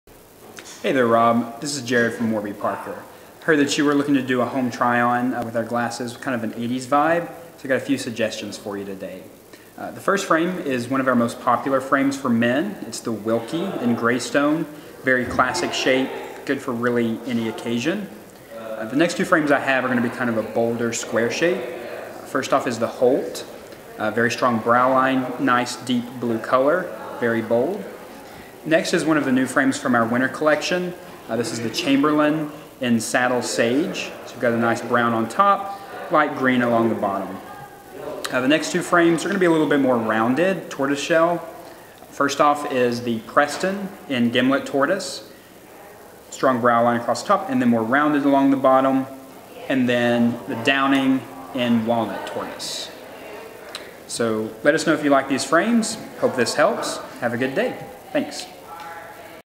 Warby Parker | Eyeglass Frames for Men
Jarred shows @RealRedwards some frames we think he might like.